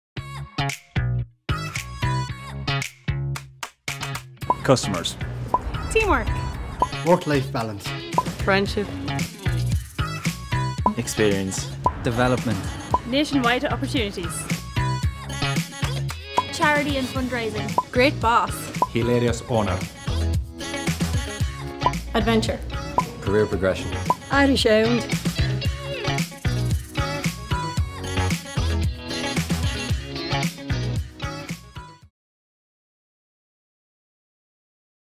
13 Reasons to Work at Mr. Price | Jobbio
Not one, not two, but 13 reasons to join the team at Mr Price! If you're convinced... check out their live roles